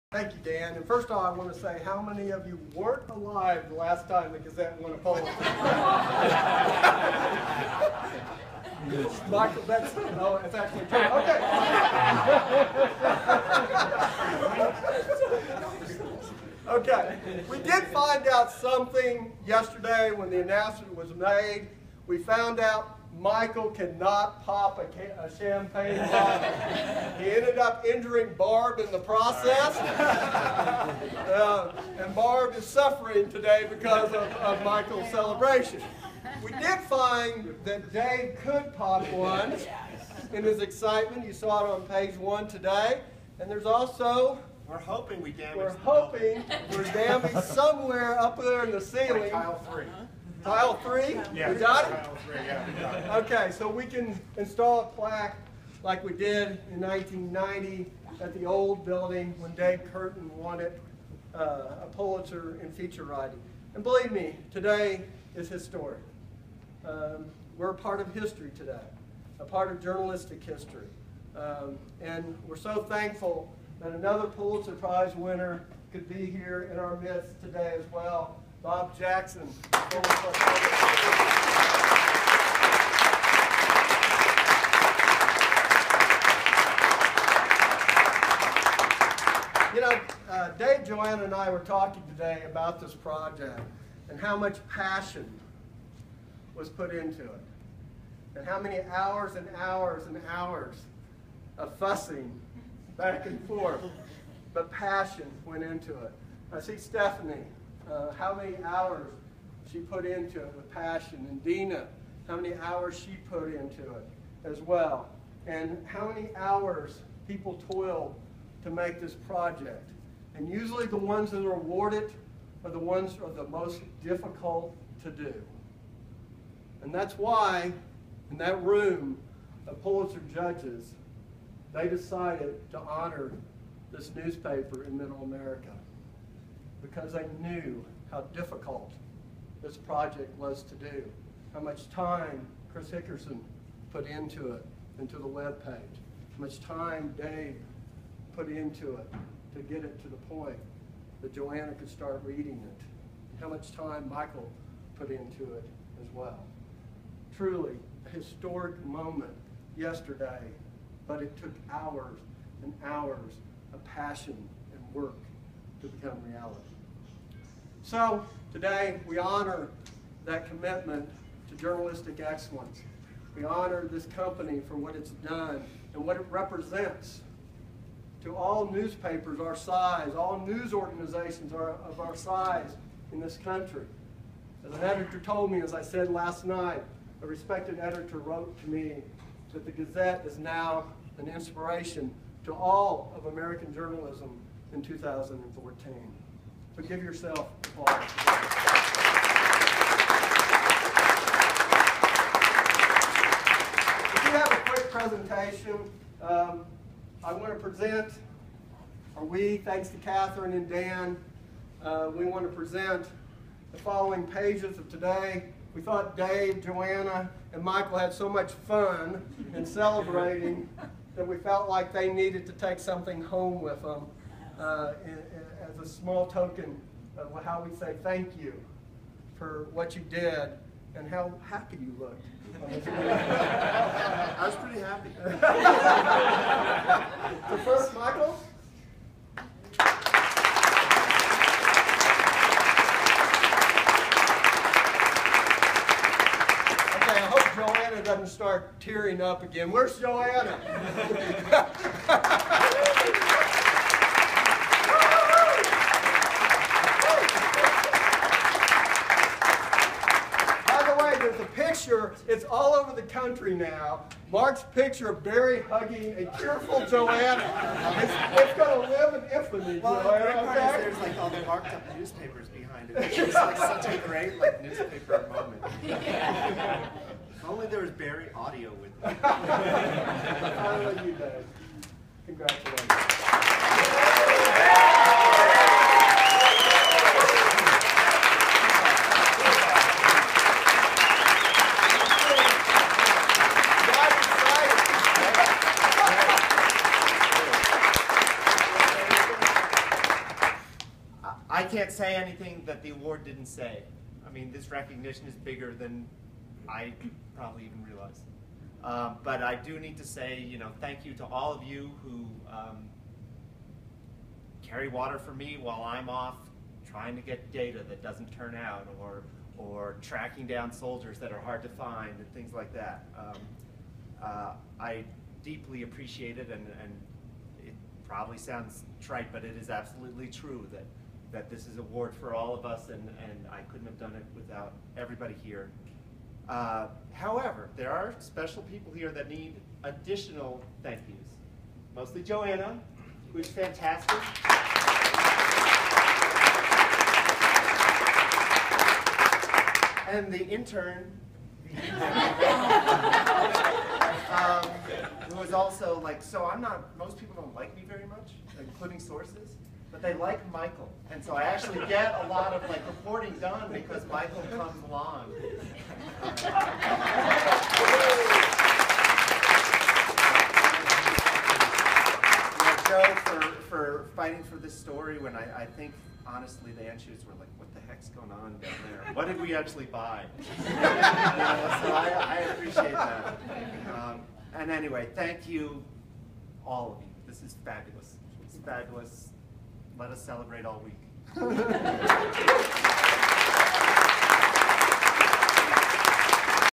Gazette Editor Joe Hight talks about the Pulitzer Prize win in a celebration Tuesday
Gazette Editor Joe Hight talks about the Pulitzer Prize win in a celebration Tuesday in the Gazette newsroom. The Gazette and reporter Dave Philipps won the award for the 'Other Than Honorable' series. Video by Dalton Walker.